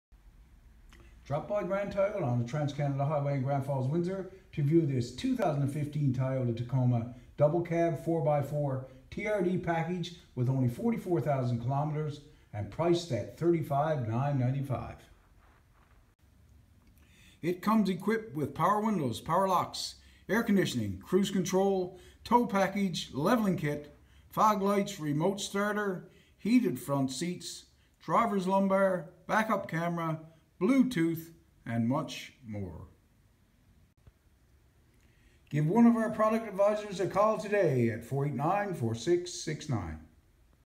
White 2015 Toyota Tacoma null Review null - Grand Toyota
This is a White 2015 Toyota Tacoma null Review null - Grand Toyota with 5-Speed A/T transmission White color and Grey interior color. (Uploaded by DataDriver).